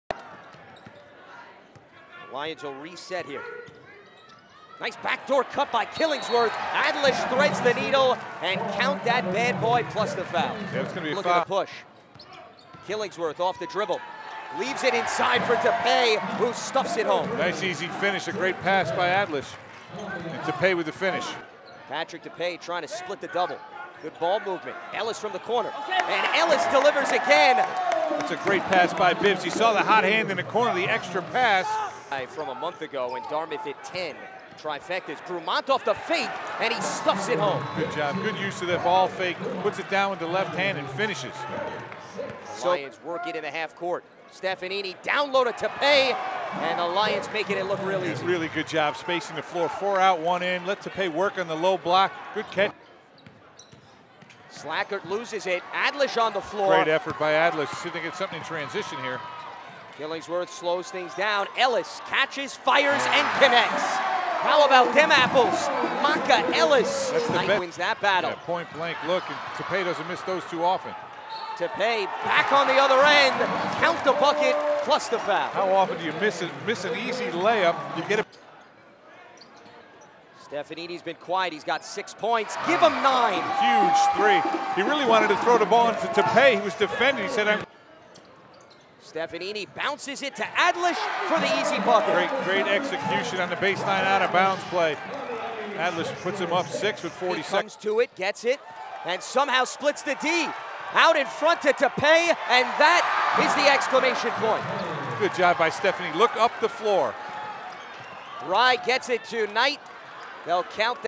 Highlights: MBK | Columbia 70, Dartmouth 66 (3/8/19)
The Columbia Lions win their third in a row, avenging an earlier-season loss to the Big Green.